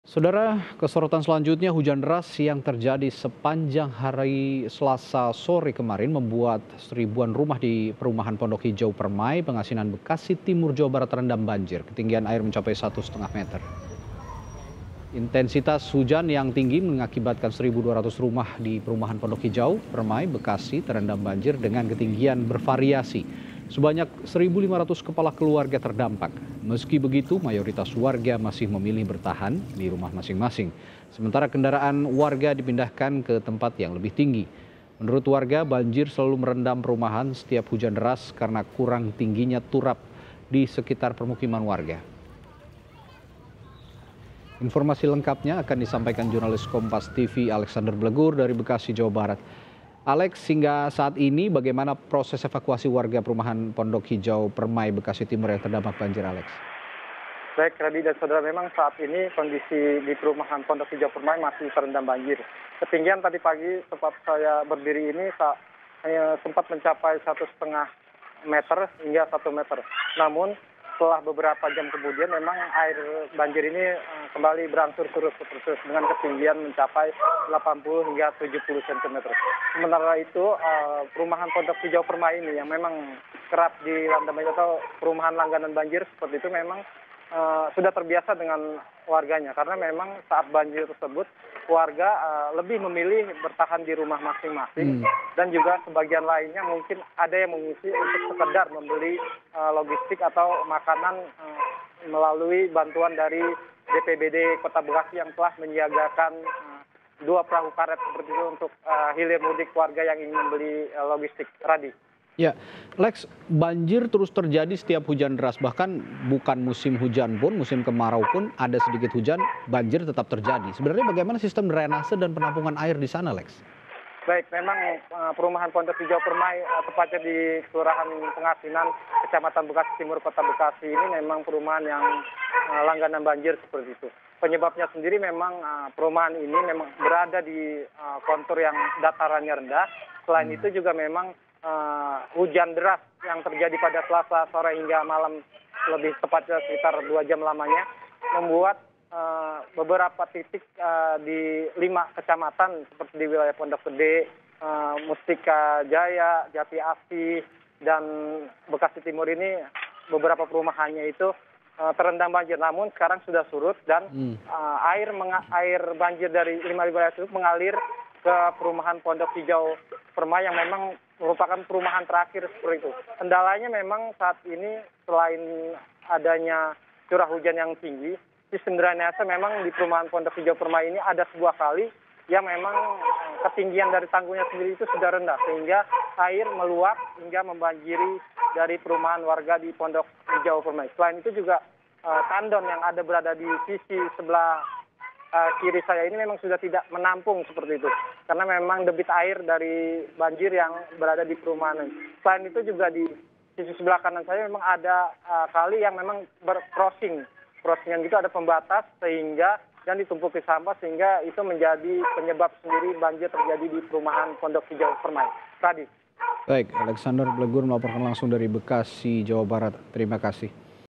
Ribuan Rumah di Perumahan Pondok Hijau Permai Terendam Banjir, 1.500 Keluarga Terdampak
BEKASI, KOMPAS.TV - Hujan deras yang terjadi sepanjang hari Selasa (15/06) kemarin, membuat lebih dari seribu rumah di Perumahan Pondok Hijau Permai, Pengasinan, Bekasi Timur, Jawa Barat, terendam banjir.

Ketinggian air mencapai 1,5 meter.

Intensitas hujan yang tinggi, mengakibatkan 1.200 rumah di Perumahan Pondok Hijau Permai, Bekasi, terendam banjir dengan ketinggian bervariasi.

Sebanyak 1.500 kepala keluarga, terdampak.

Meski begitu, mayoritas warga masih memilih bertahan di rumah masing masing.

Sementara, kendaraan warga dipindahkan, ke tempat yang lebih tinggi.

Menurut warga, banjir selalu merendam perumahan, setiap hujan deras, karena kurang tingginya turap di sekitar permukiman.